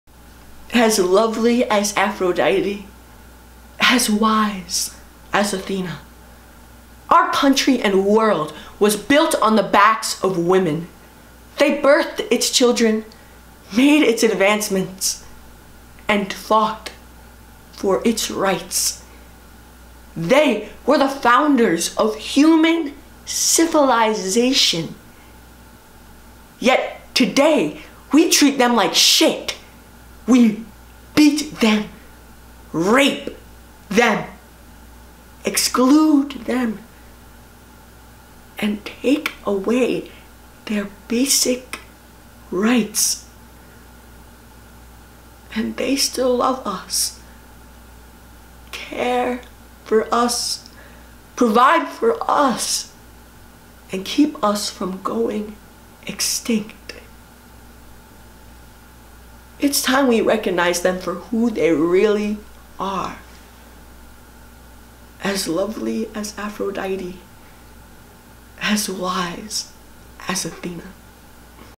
Women - written and performed by Julian S.
An eloquent description of women's worth.